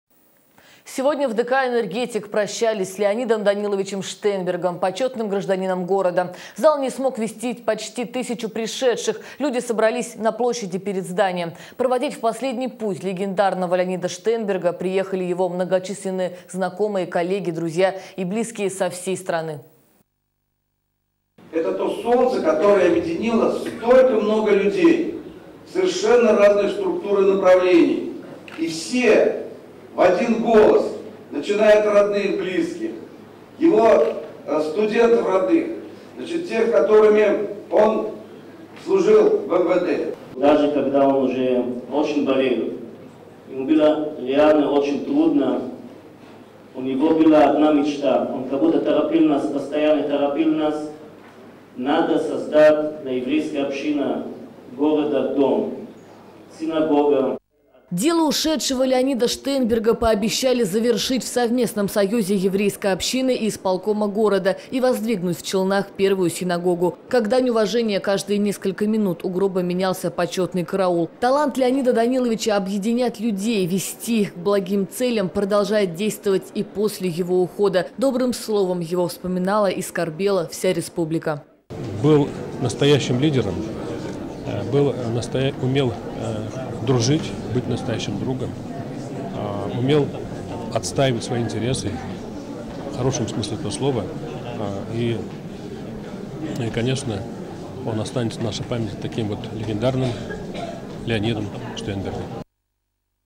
«Он останется в нашей памяти таким легендарным Леонидом Штейнбергом»
Сегодня в ДК «Энергетик» прощались с Леонидом Даниловичем Штейнбергом – почетным гражданином города. Зал не смог вместить почти тысячу пришедших – люди собрались и на площади перед зданием. Проводить в последний путь легендарного Леонида Штейнберга приехали его многочисленные знакомые, коллеги, друзья и близкие со всей страны.